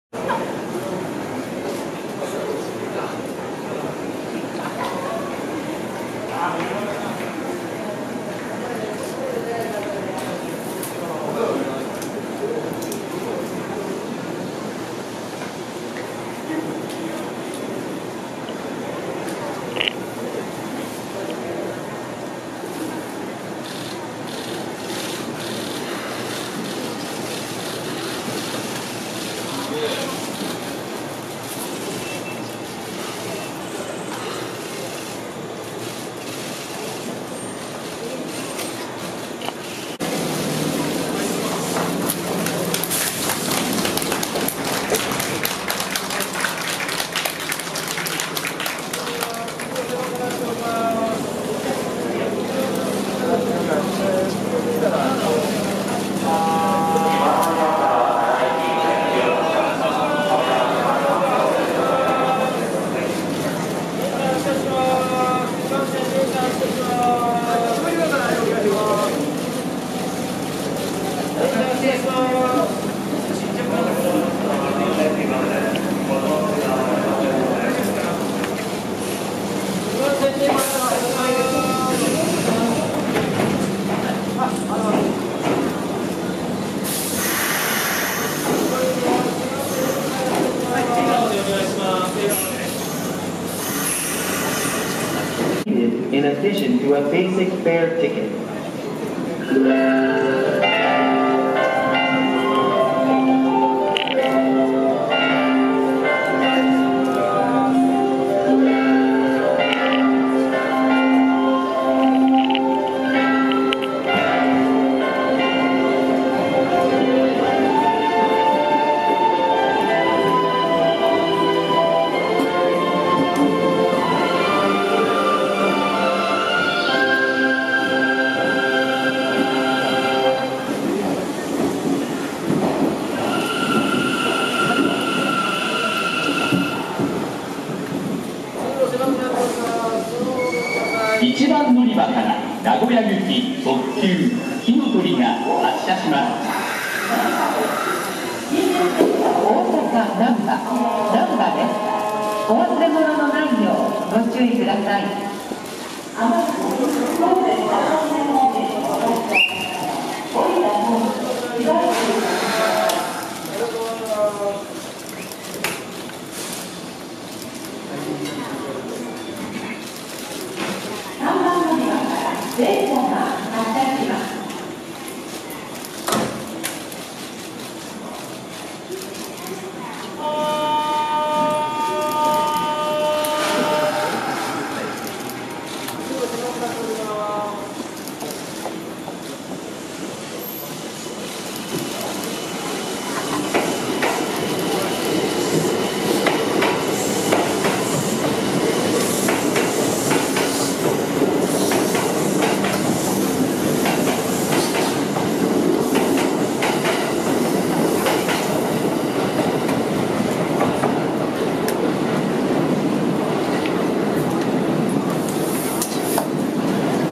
近鉄名阪特急ひのとり一番列車、大阪難波駅出発式の模様
2020年3月14日(土)近鉄大阪難波駅にて